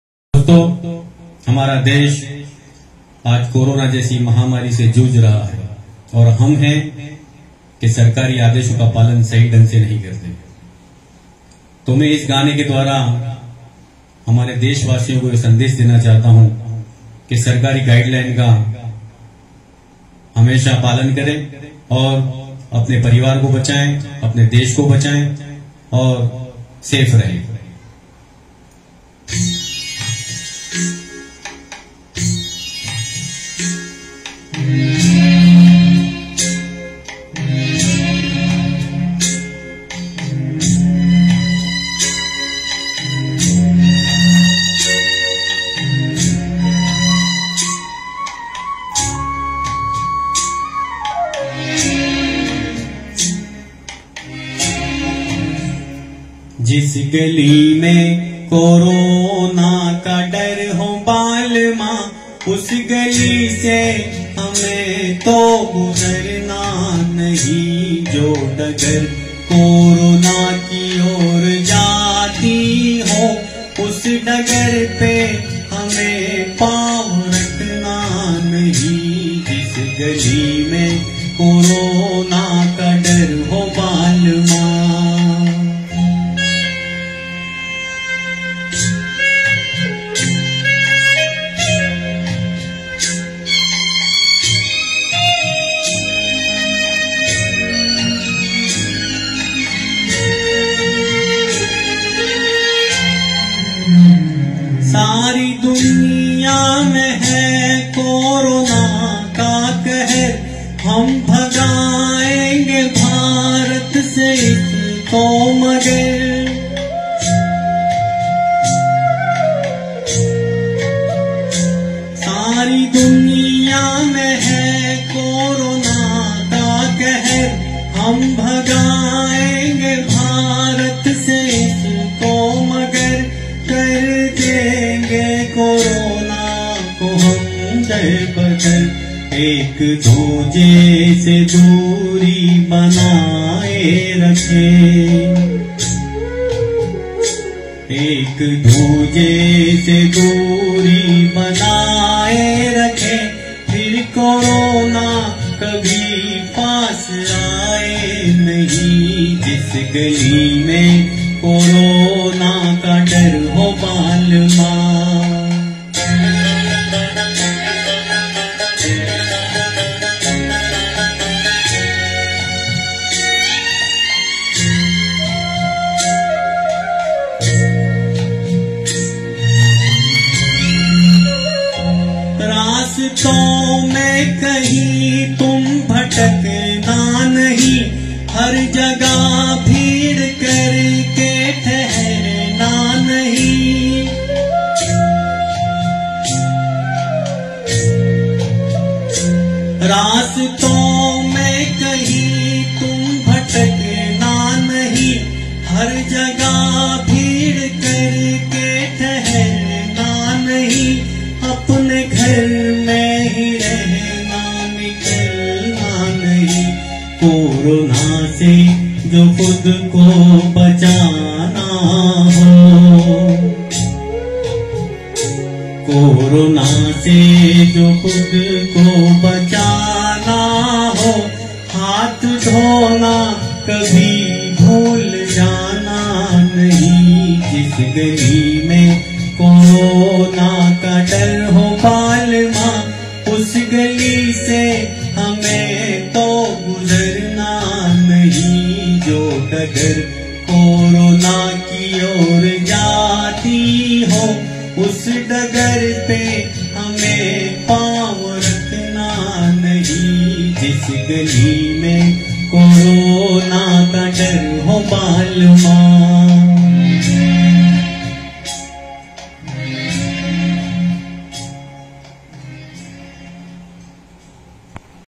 Corona Song Originally Sung By Mukesh
Self Composition To Make people aware about Corona